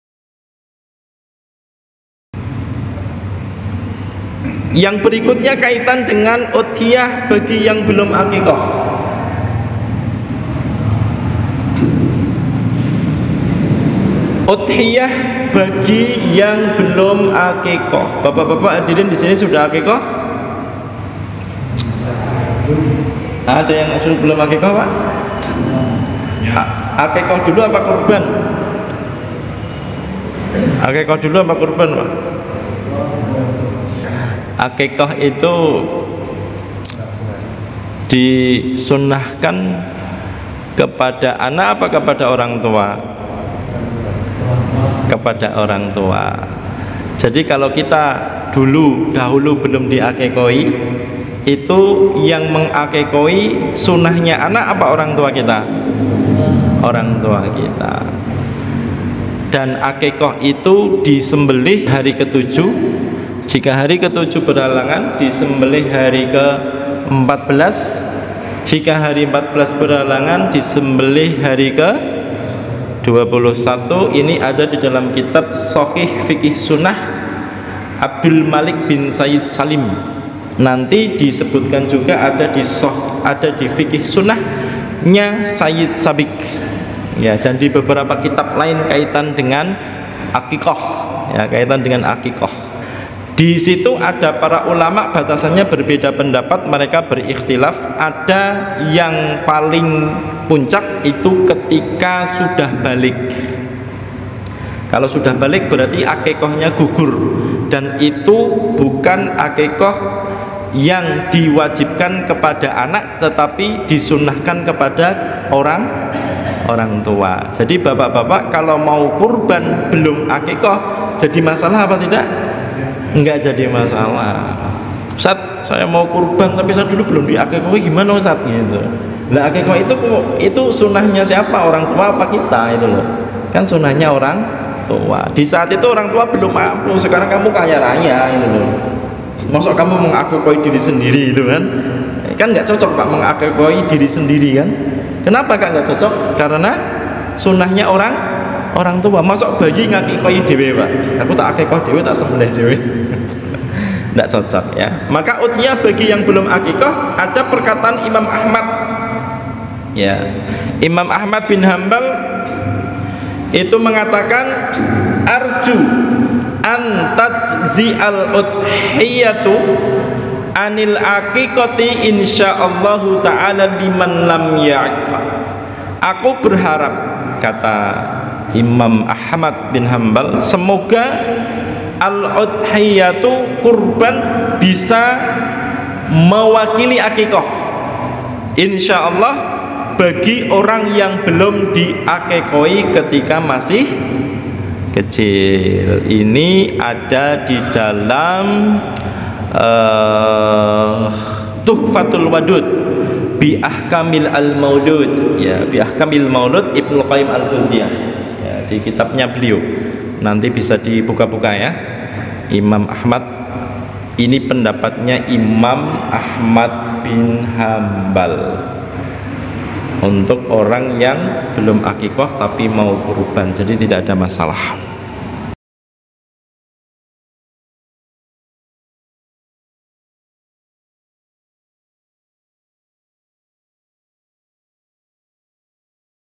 Hukum Berkurban Belum Aqiqah || Ustadz Arif Mushlih
Hukum Berkurban Belum Aqiqah

Sebagian dari kita mungkin ada yang ingin berqurban tetapi dulu belum diaqiqah, ketika sudah dewasa dan sudah mampu kita bingung bolehkah jika kita ikut berqurban padahal belum aqiqah.

Apa hukumnya berqurban bagi yang belum aqiqah?
Apakah hukum aqiqah masih boleh ketika kita sudah dewasa?
Siapa sebenarnya yang disunnahkan untuk beraqiqah apakah orang tua atau boleh sang anak?

Yuk simak penjelasan berikut oleh :
Ustadz Arif Mushlih

barakallahu fiekum
semoga bermanfaat